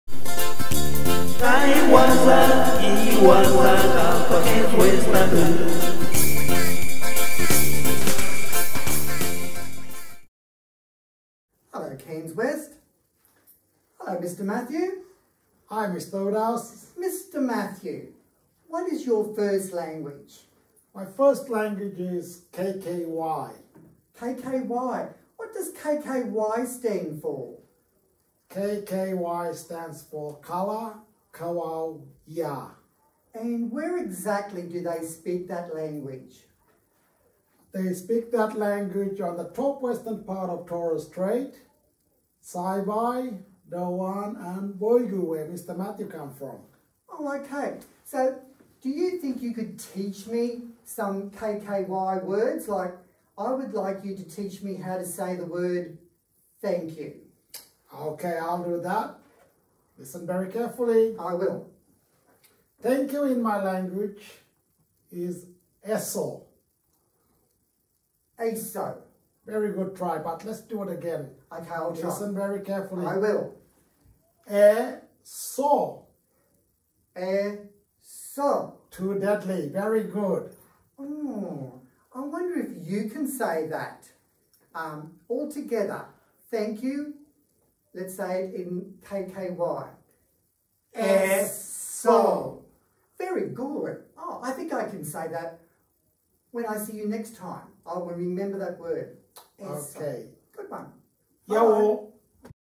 KKY #1
Mr Matthew teaches Mr Woodhouse some words in Kalaw Kawaw Ya (KKY).

KKY is the traditional language of the people from the islands of Saibai, Dauan and Boigu from the Top Western islands of the Torres Strait. It is spoken by a number of families at Cairns West State School.